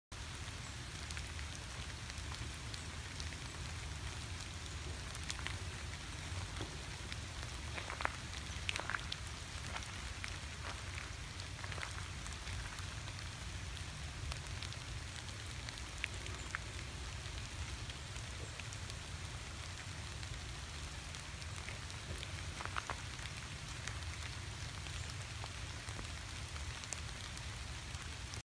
OxleyArmadillo
This little guy crossed the trail right next to me when I stopped to watch the field in the rain on my walk out at Oxley Nature Center.